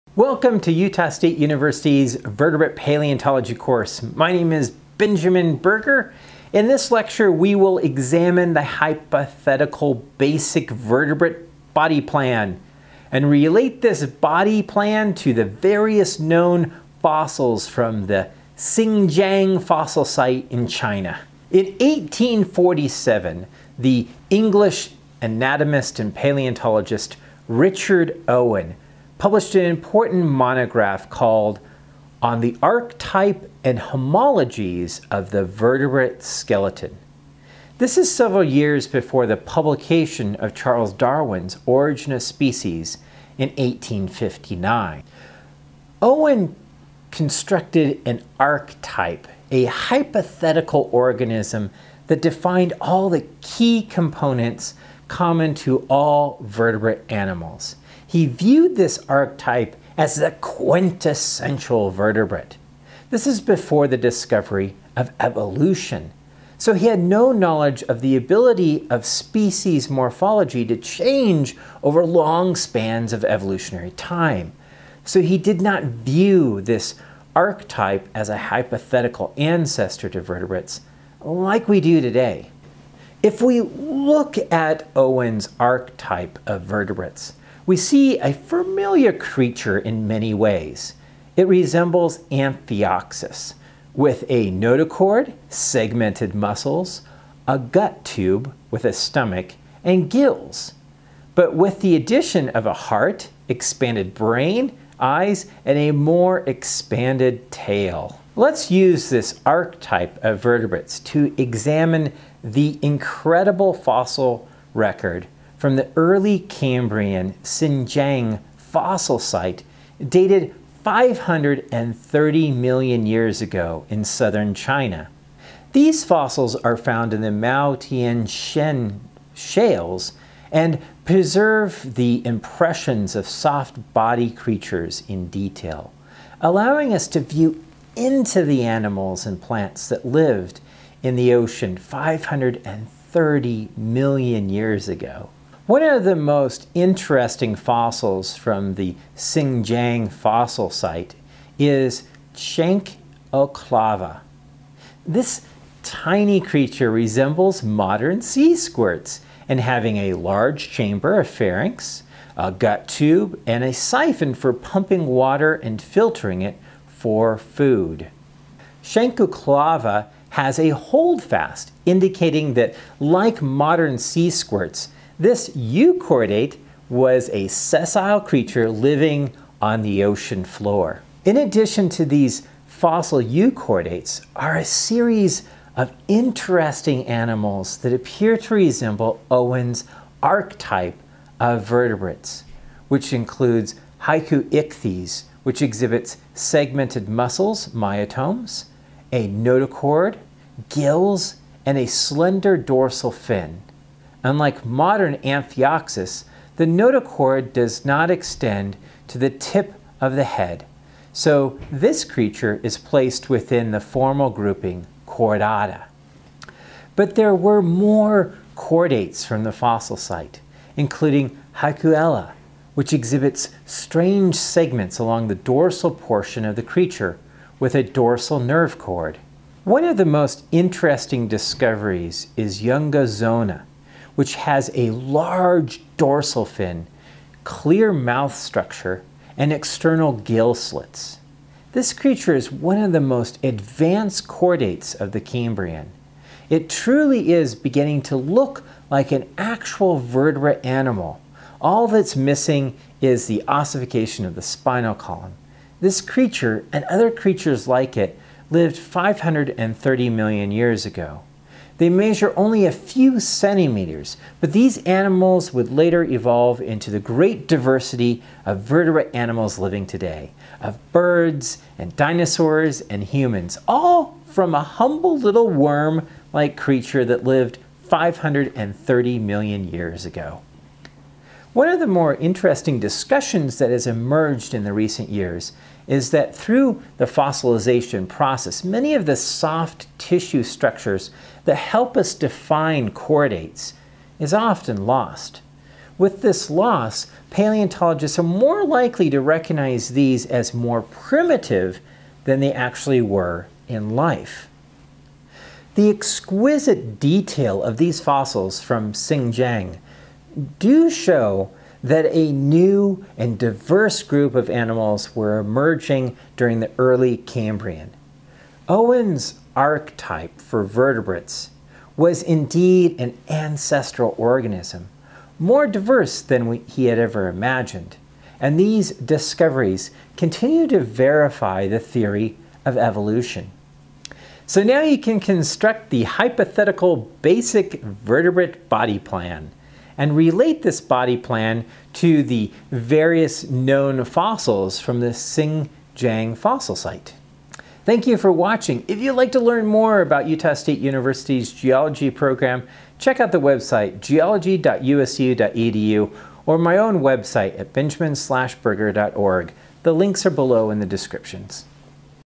Lecture 6 Owen's Body Plan and the Fossils of Chengjiang
In this lecture you will learn how to construct a hypothetical “basic” vertebrate body plan, and relate this body plan to the various known fossils from the Chengjiang fossil site.

Other images come from the textbook: Benton, M.J. 2014. Vertebrate Paleontology fourth edition. Published by Wiley and Blackwell Publishing.